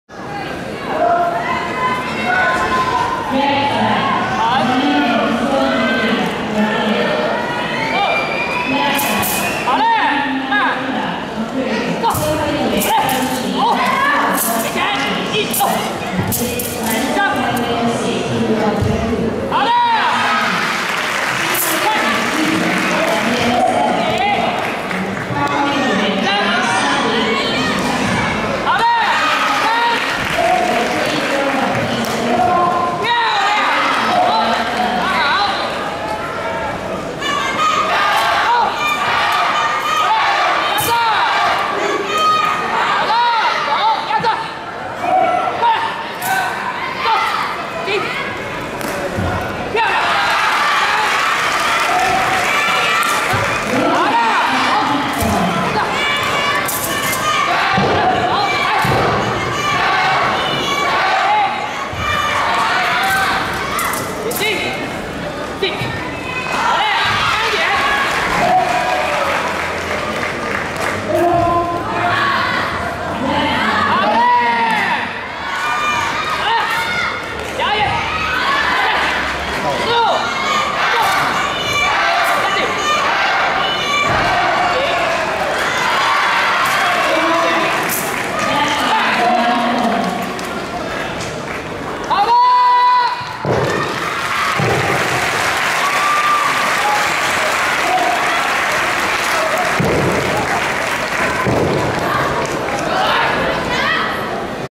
12th World Wushu Championships, Women Jianshu MAS Phoon Eyin 潘依嫣 9.50
2013 12th World Wushu Championships, Kuala Lumpur, Malaysia. Women Jianshu 女子剑术 Malaysia Phoon Eyin 潘依嫣 9.50